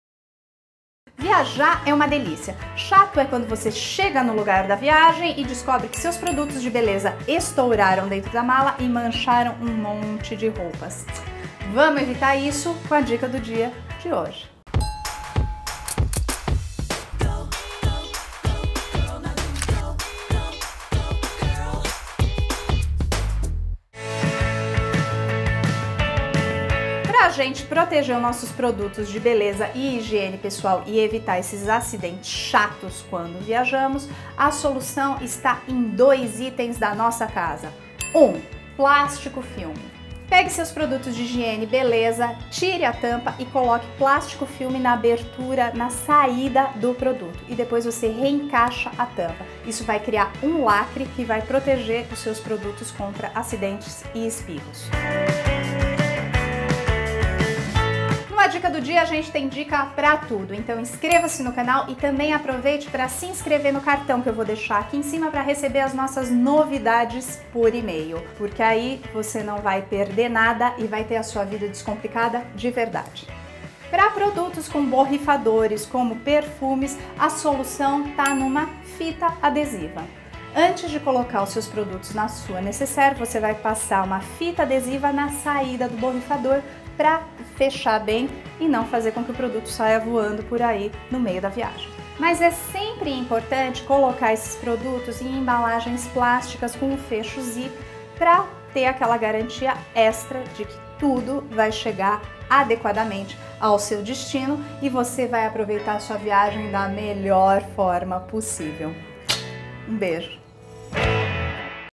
COMO ORGANIZAR COSMÉTICOS PARA VIAGEM | A DICA DO DIA COM FLÁVIA FERRARI
SEU LIVRO DO A DICA DO DIA Veja as dicas que vão evitar que shampoos, condicionadores, cremes, perfumes e demais itens da sua viagem derramem, sujando suas roupas, sua mala e criando aquela confusão.